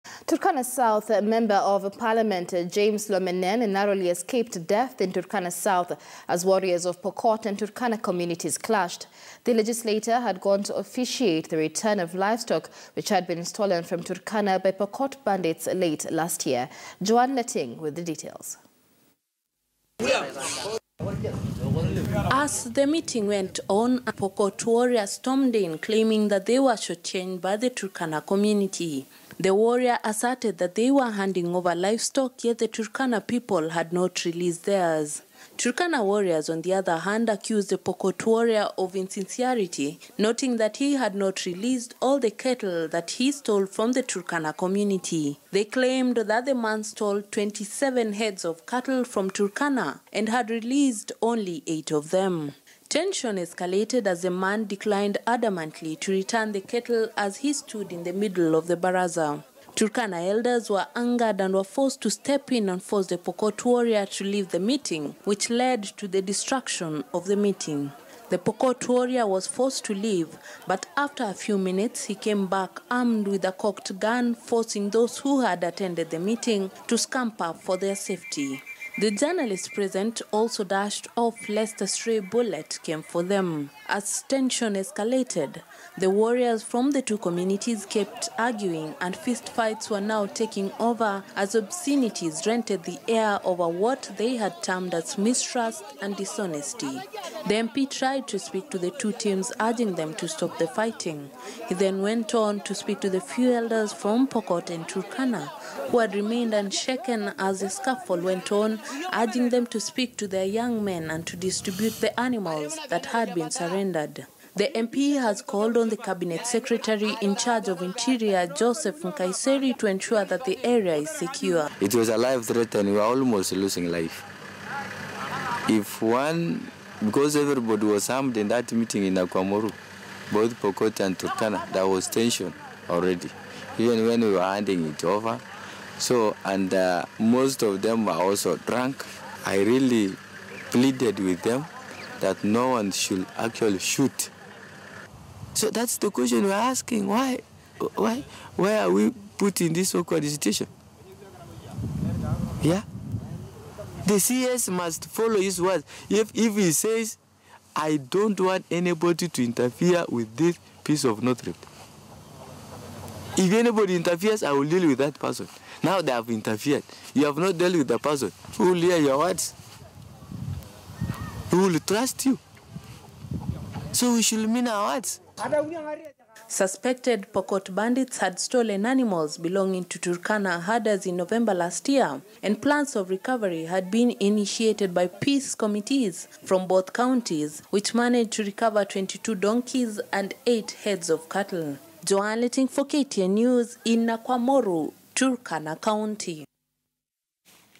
Turkana South MP narrowly escapes death as Pokot-Turkana warriors clash
Turkana South Member of Parliament James Lomenen narrowly escaped death in Turkana South as warriors of Pokot and Turkana communities clashed. The legislator had gone to officiate the return of livestock which had been stolen from Turkana by Pokot bandits late last year.